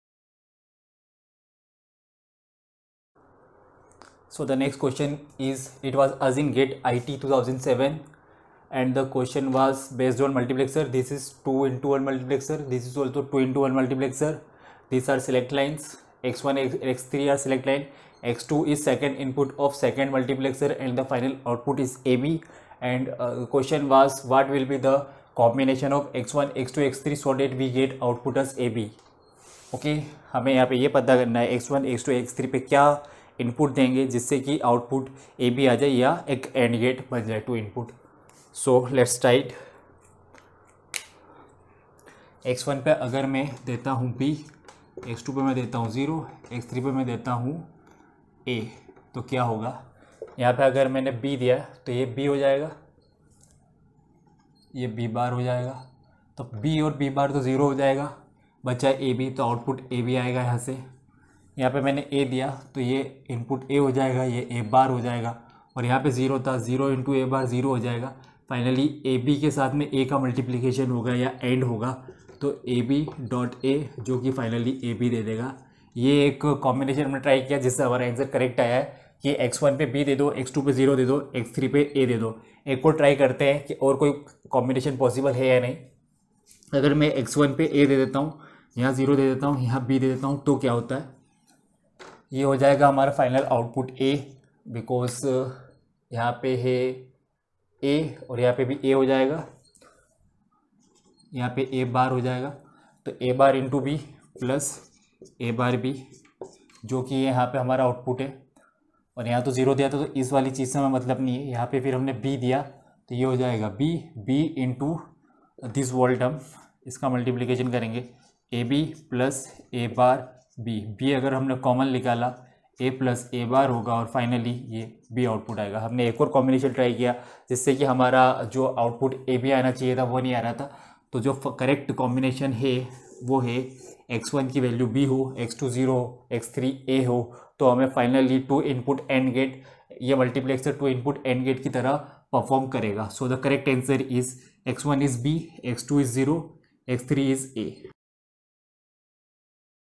3.6 Multiplexers - GATE Previous Year Questions - Digital Logic
In this video we will solve gate previous year questions on Digital logic